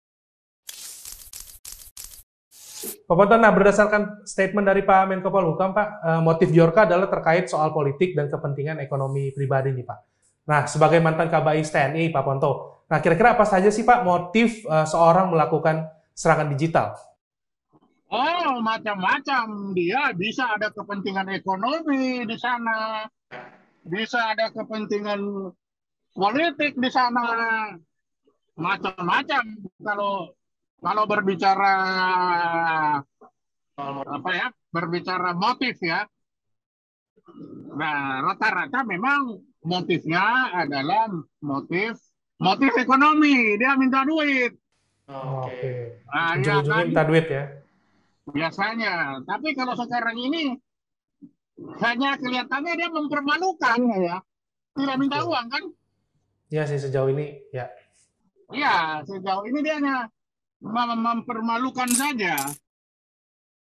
Eks Kabais TNI Sebut Motif Bjorka Hanya Mempermalukan
Menteri Koordinator Bidang Politik, Hukum, dan Keamanan (Menko Polhukam) Mahfud MD mengungkapkan motif Bjorka dalam melakukan serangan siber terkait dengan politik dan kepentingan ekonomi pribadi. Hal itu disampaikan Mahfud MD di kantornya, Jakarta Pusat, pada Rabu (14/9/22).

Menanggapi hal itu, Mantan Kepala Badan Intelijen Strategis TNI, Soleman B Ponto menjelaskan ada berbagai jenis motif seseorang melakukan serangan siber. Ponto mengatakan rata-rata motif seseorang melakukan serangan siber terkait kepentingan ekonomi. Namun, melihat motif serangan siber yang dilakukan oleh sosok peretas yang mengatasnamakan dirinya Bjorka hanya untuk mempermalukan.